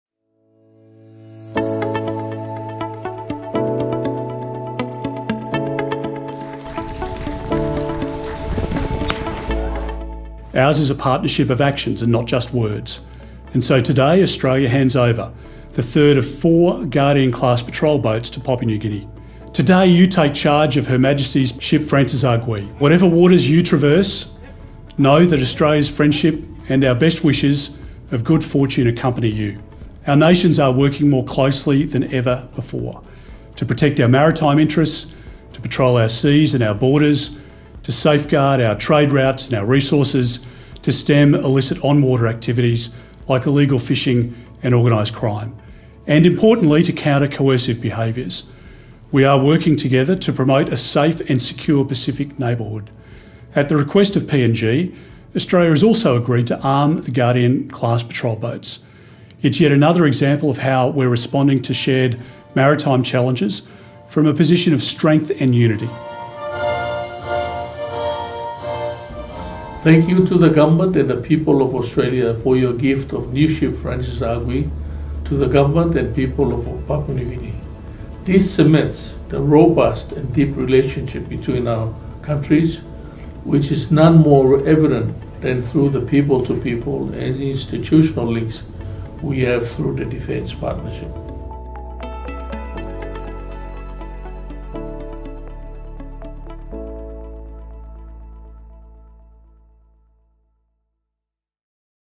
Guardian-class Patrol Boat presented to Papua New Guinea
Papua New Guinea took receipt of the third of four Guardian-class Patrol Boats delivered under the Pacific Maritime Security Program.

The handover ceremony took place in Henderson, Western Australia with Papua New Guinea High Commissioner, His Excellency Mr. John Kali, via live stream from Canberra, accepting possession of HMPNGS Francis Agwi on behalf of PNG.

Australia is working with Pacific partners to enhance the maritime capability to maintain a secure, free and open Pacific.